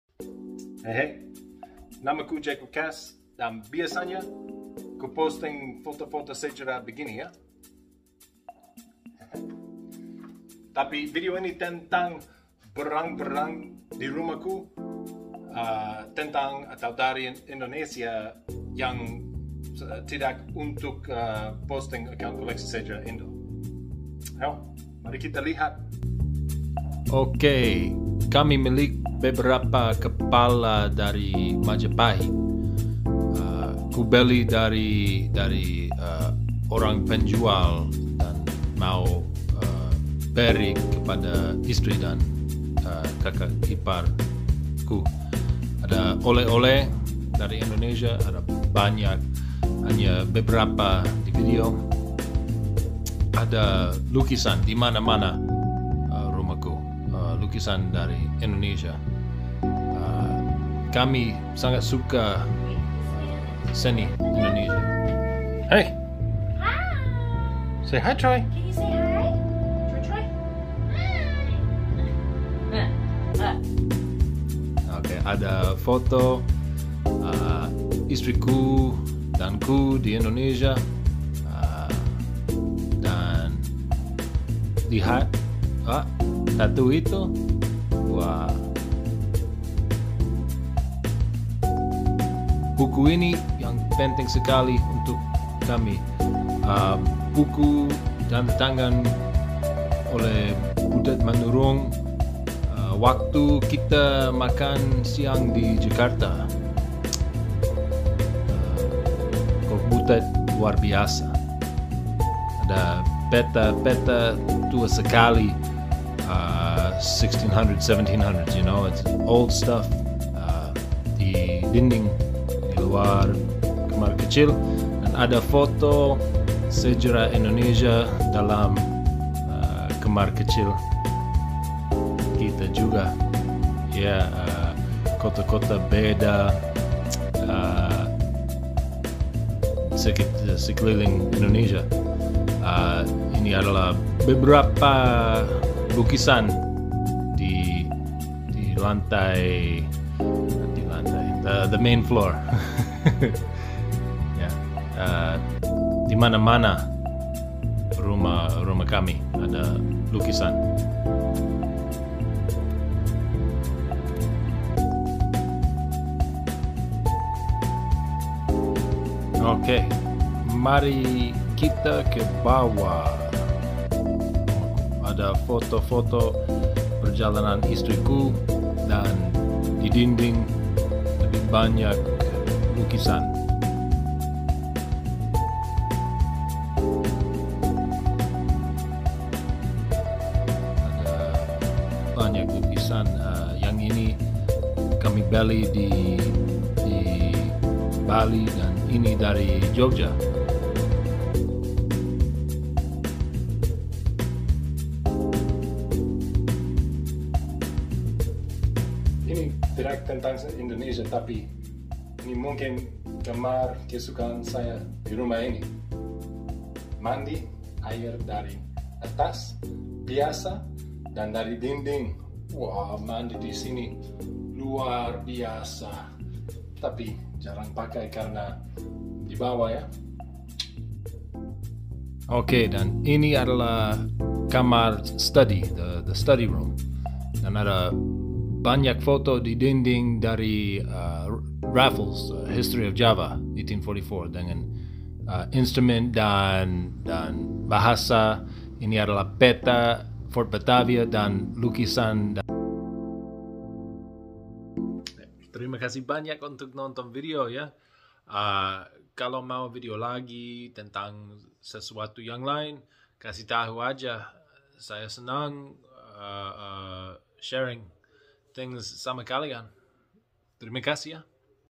Barang2 Indonesia di rumah.
Ini adalah barang2 tentang atau dari Indonesia yang saya tidak akan post untuk koleksi_sejarah_indo.